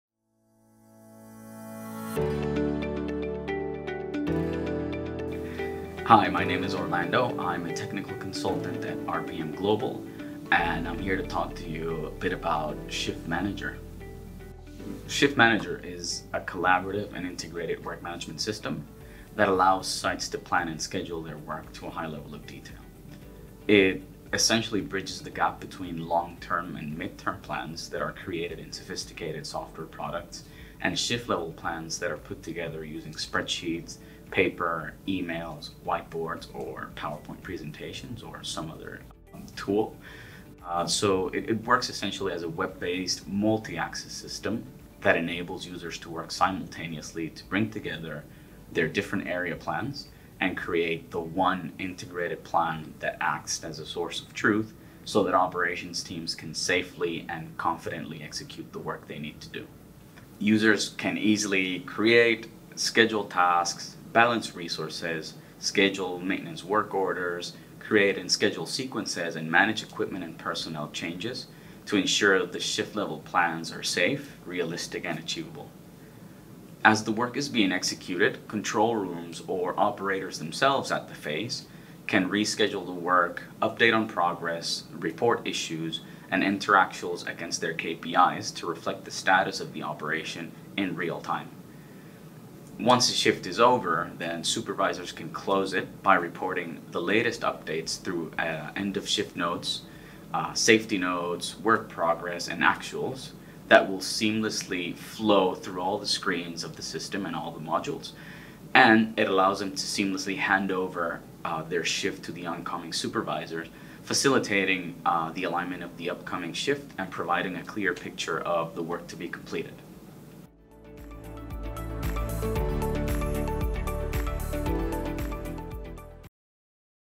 ShiftManager - One plan for all
RPMGlobal’s ShiftManager is a real-time short term planning and execution solution allowing every task to be planned, measured and reported. It provides transparency for all users as information is available to all departments through one integrated web-based plan. Available online, offline, and as a mobile platform, ShiftManager enables mining organisations to plan, measure, and improve every task.